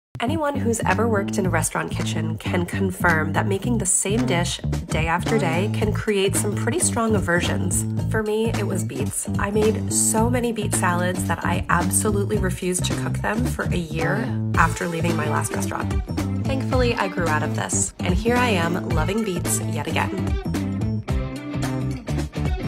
Beet Salad with Goat Cheese & Pine Nuts
My Beet Salad with Goat Cheese and Pine Nuts has just 7 ingredients and is the perfect side dish or salad for any Middle Eastern meal. Not only is the combination of beets, creamy goat cheese, and crunchy toasted pine nuts delicious, but it's absolutely stunning too -- it reminds me of a bejeweled salad. All it needs is a drizzle of a nice extra virgin olive oil to bring it together. Easy elegance! Gluten-free.

🍴 Ingredients🍴
4-5 medium red beets, though any variety of beets will work
1 tsp Kosher salt
½ cup pine nuts
High quality extra virgin olive oil
3 Tbsp fresh dill
2-4 ounces goat cheese
2 tsp fresh lemon zest, about 1 medium lemon
Flaky sea salt

♥️ BE SURE TO LET US KNOW IF YOU MADE THIS AT HOME! ♥️